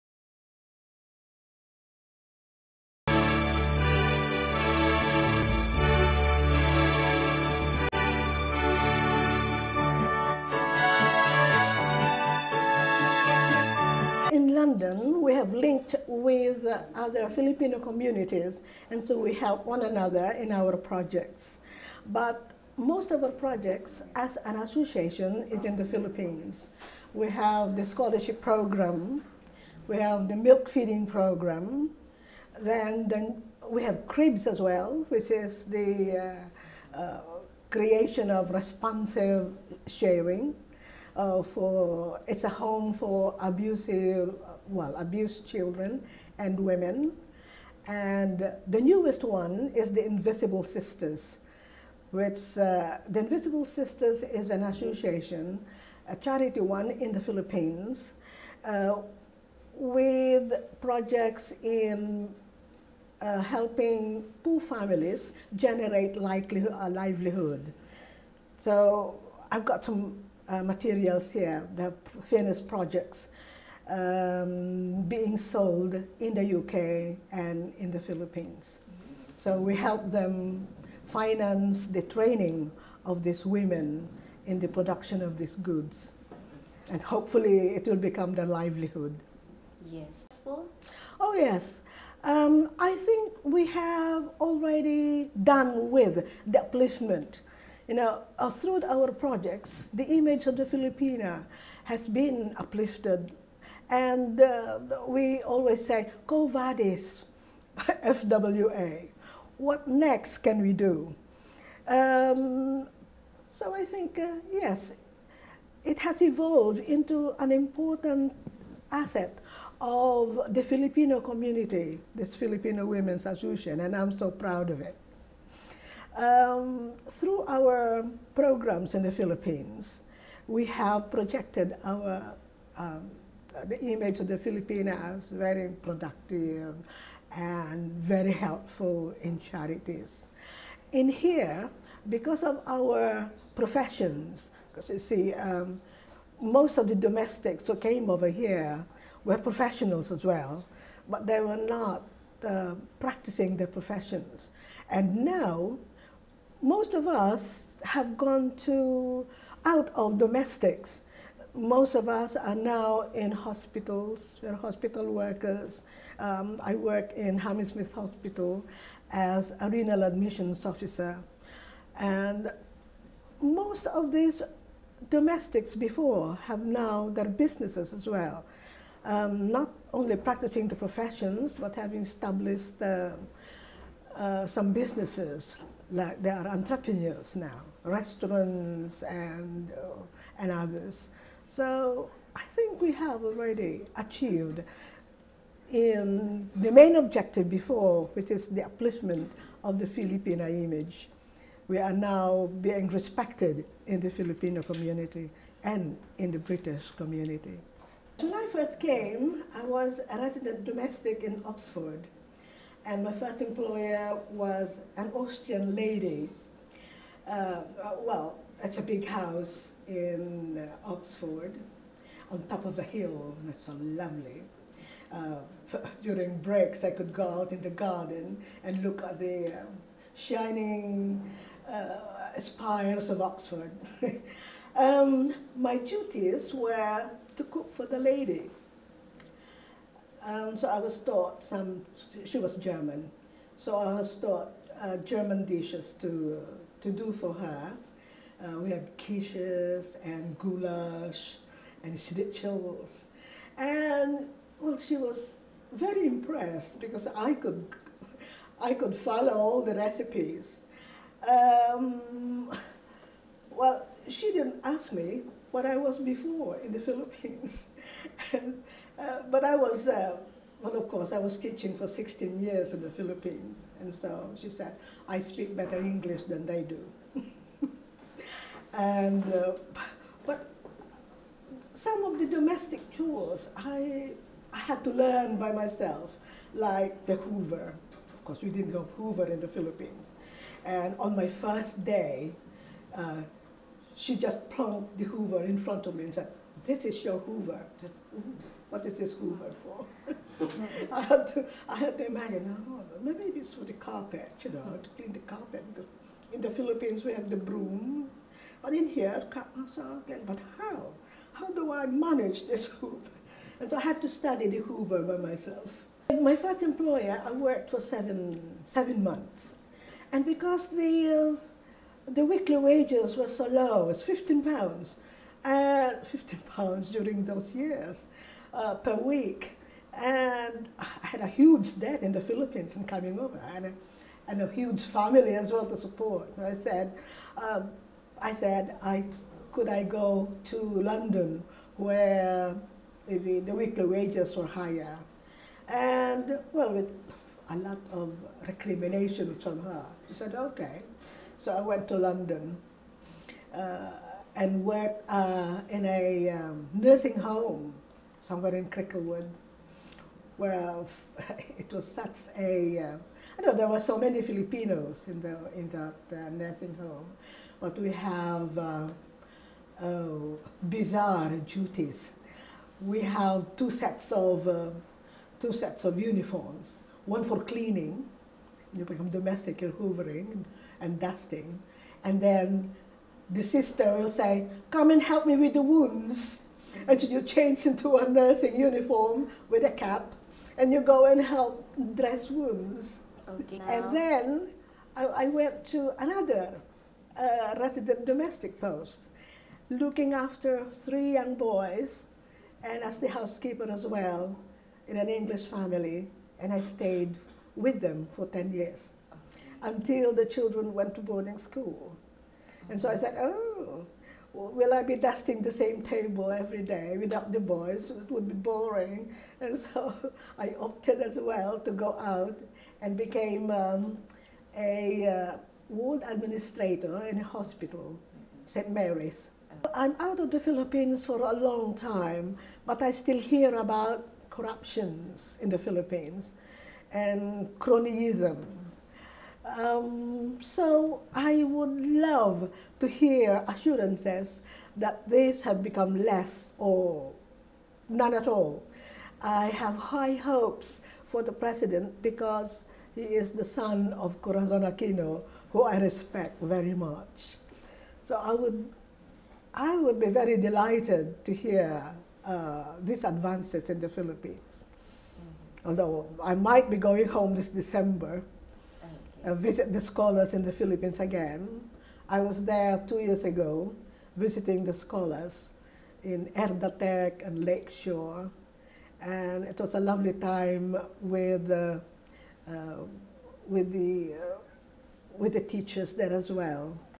Interview with Nievecitas Martin, Current Chairman of FWA U.K. - 3/6/2012
3 June 2012
London